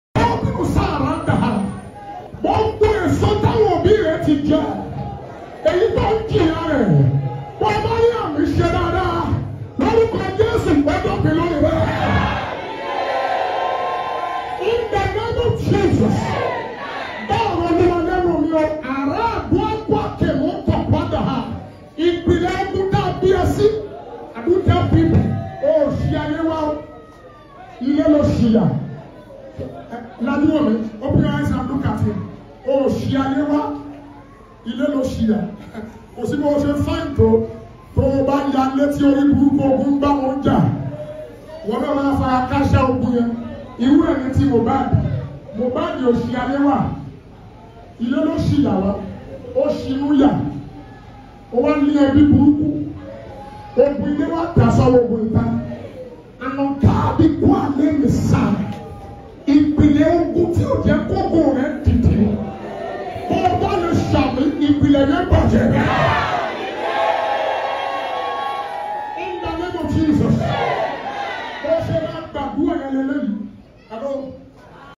FOUNDATIONAL BATTLE IS DESTROYED
Every foundational battle that's impeding and obstructing your glory is totally uprooted today! You are not here accidentally, but you were born in a problematic household. Therefore, as the Lord lives and His Spirit lives, your battle is over. Your glory will rise and shine in Jesus' mighty name!